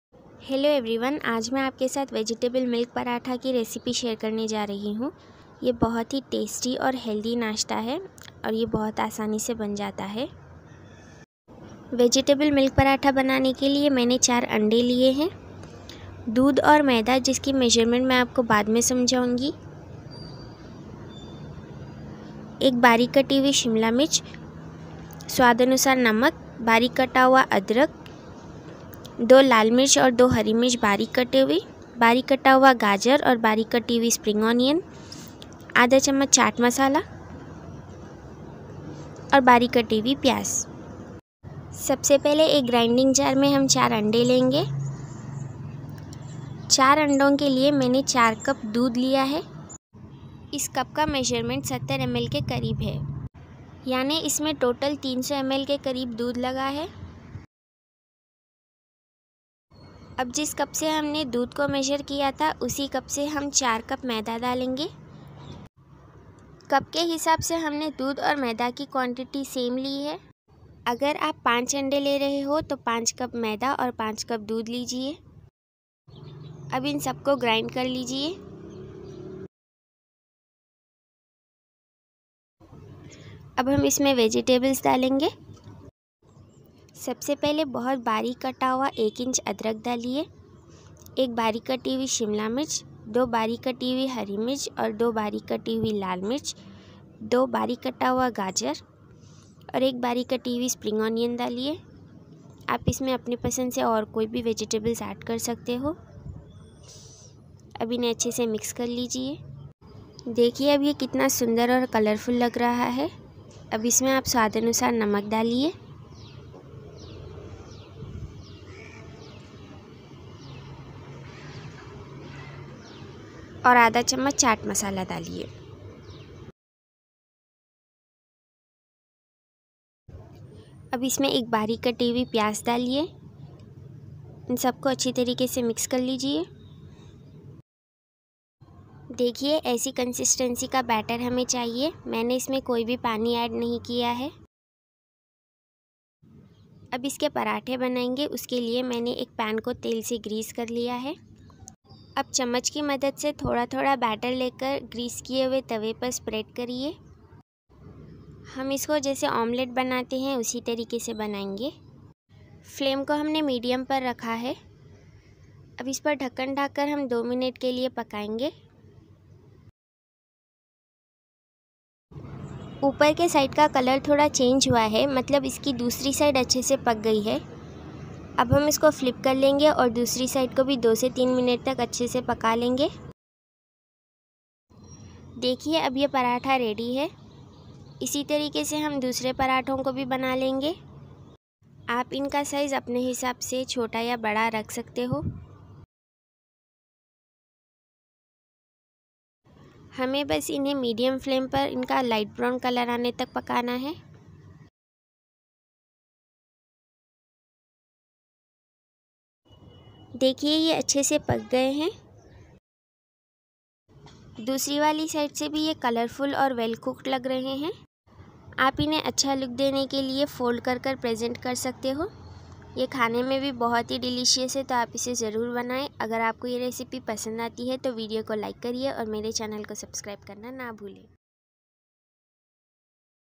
Vegetable milk omelette paratha recipe for breakfast/ tasty and healthy recipe/ पराठा रेसिपी
Vegetable milk omelette paratha recipe

Ingredients:
Eggs=4
Milk=4 cups
Maida=4 cups
Ginger=1 inch
Green chillies=2
red chillies=2
Capsicum=1
Carrot=2
Onion =1
Chat masala
Salt
Spring onion=1

Happy cooking, enjoy eating 😊 ❤️